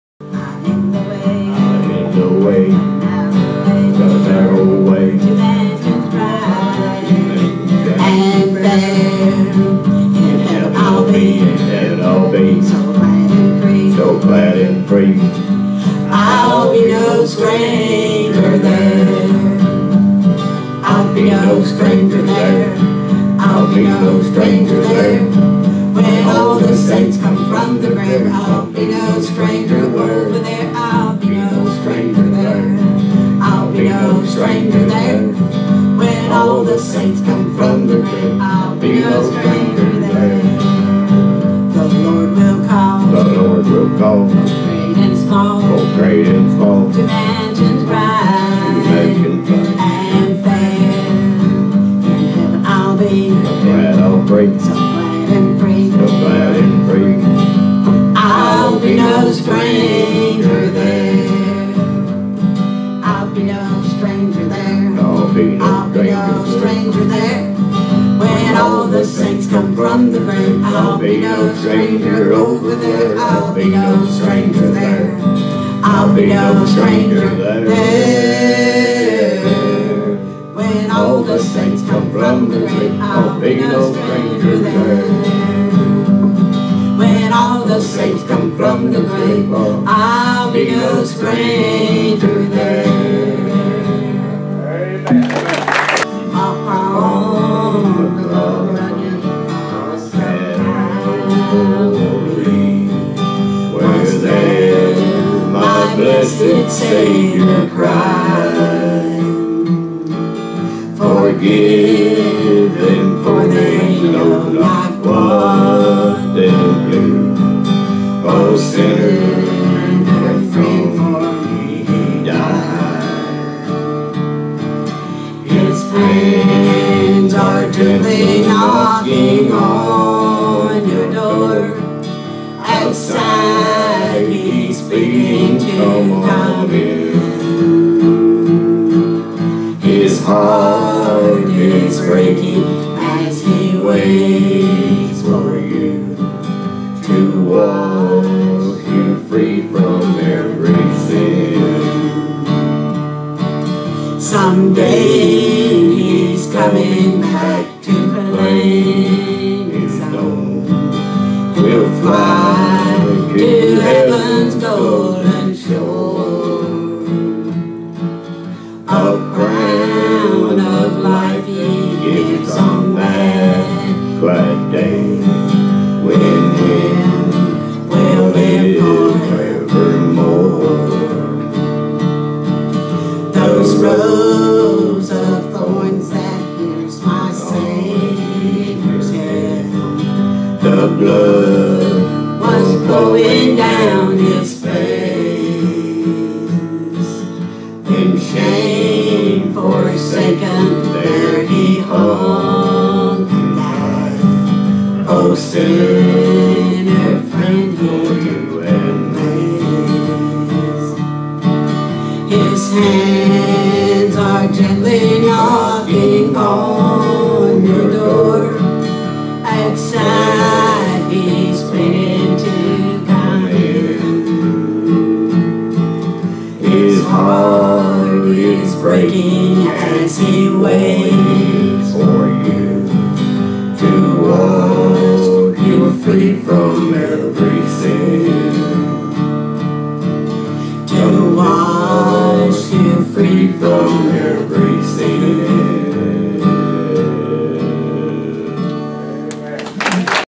2015 11 15 AM Rachel & Jack Snider with Reggie Lanoue
Rachel & Jack Snider with Reggie Lanoue sing” on November 15, 2015 at Friendship Baptist Church in Sun City Center, Florida.  Visit the Church website at friendshipbaptistchurchscc.com